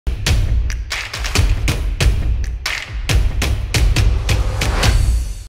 Smart Eco-MediGlo (5 Sec)
Smart Eco-MediGlo
1st Truly Malaysian Anti-Viral Paint (Mid Sheen Finish)

Effective Against Viruses & Bacteria for more than 99.9% !